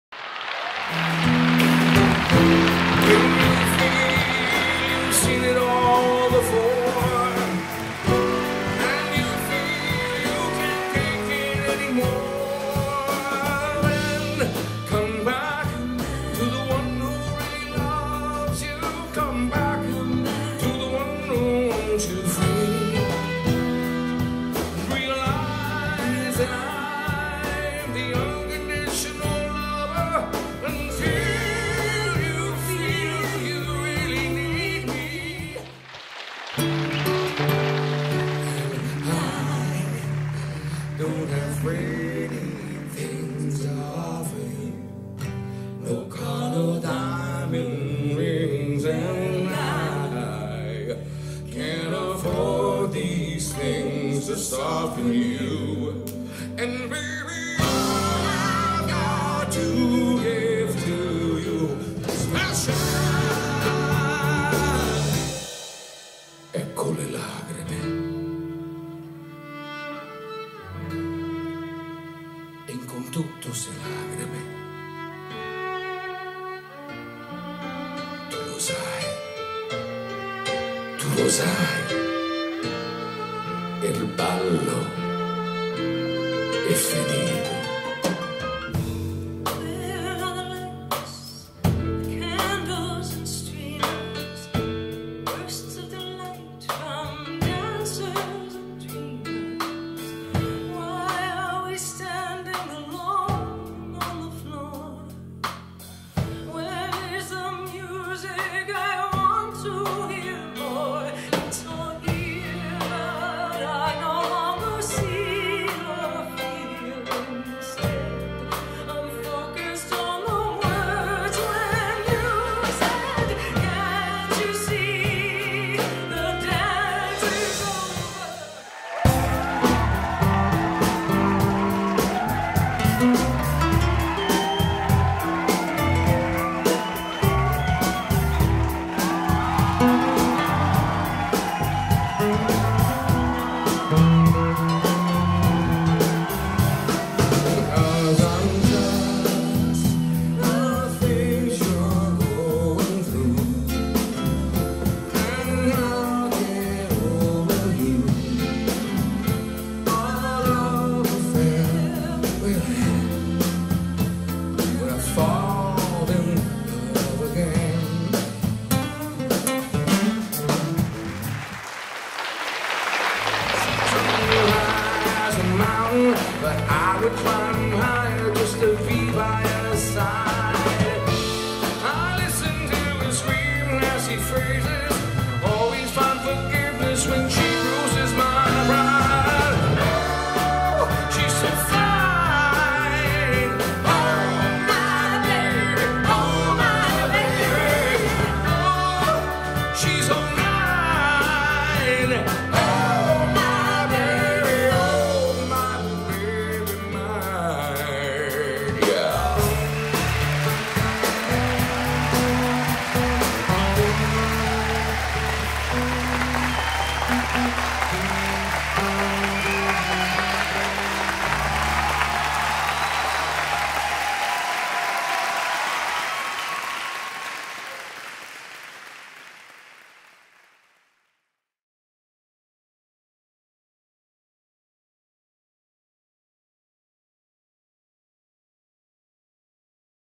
CabaRomantico trailer - starring Alfie Zappacosta and Janet MacEwen
CabaRomantico - an intimate concert/cabaret written, conceived by and starring Janet MacEwen and Alfie Zappacosta and featuring passionate selections from Juno and American Music Award winning singer/songwriter Zappacosta's impressive song catalogue. Produced by Delphi Productions in association with Janet MacEwen and Alfie Zappacosta.
The band: Ross Wooldridge - MD on keys,  Claudio Vena - accordion and viola, Russ Boswell - bass, and Blake Manning on drums.